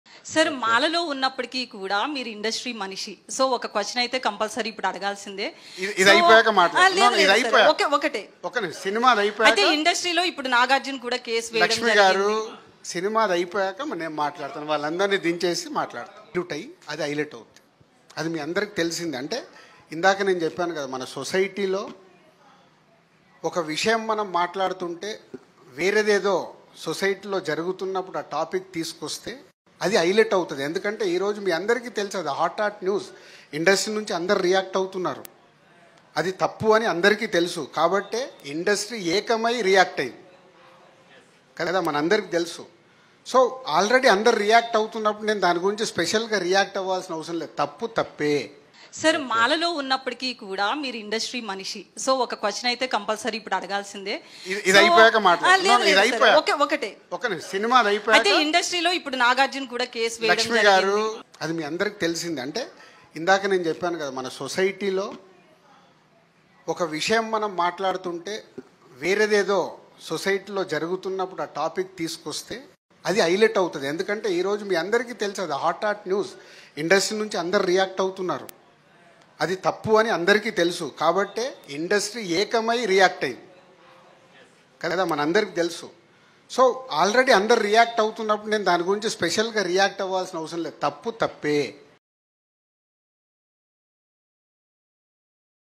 ఇండస్ట్రీ దమ్మెంటో చూపిస్తాం.. వణికి పోవాలి.. Dil Raju Sensational Comments on Konda Surekha | Dial
ఇండస్ట్రీ దమ్మెంటో చూపిస్తాం.. వణికి పోవాలి.. Dil Raju Sensational Comments on Konda Surekha | Dial News